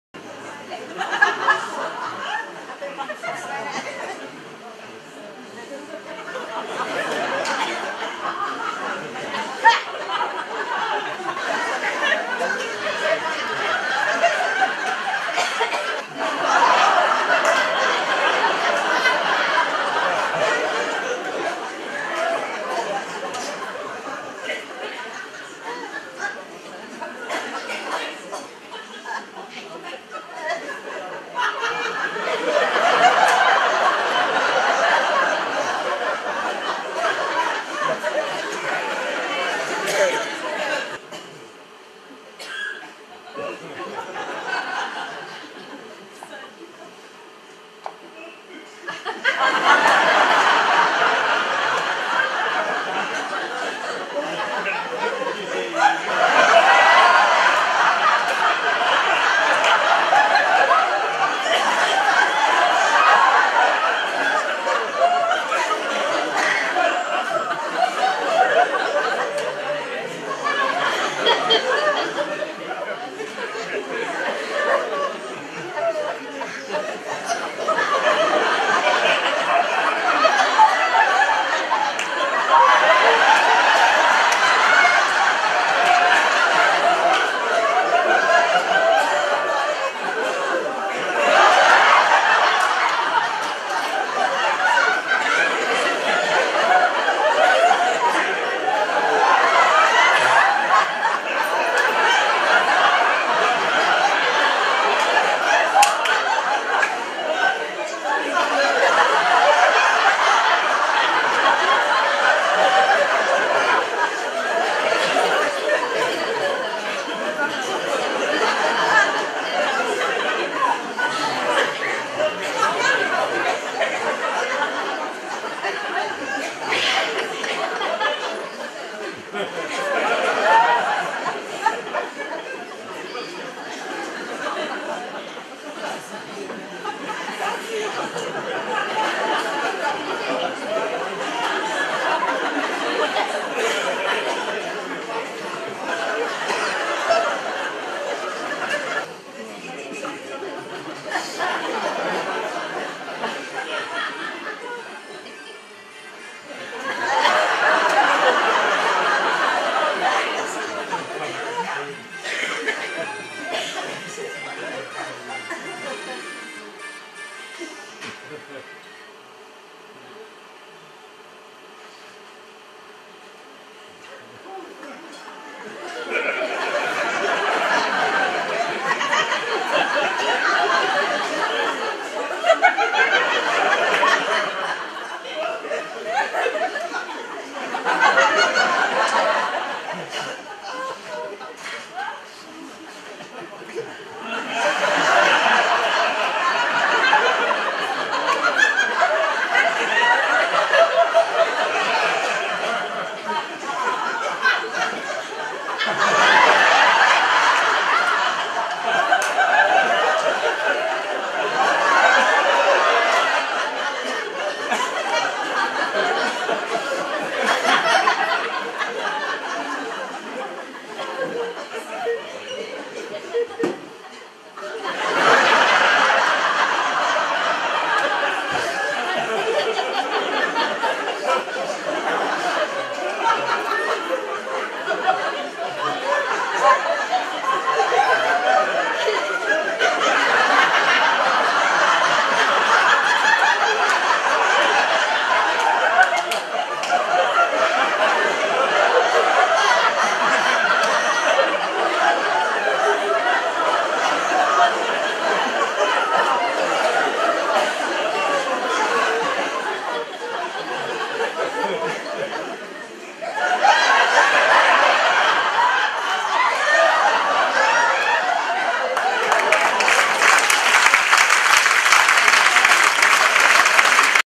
Une franche camaraderie lorsque les hommes se retrouvent aux toilettes...
Sketch urinoir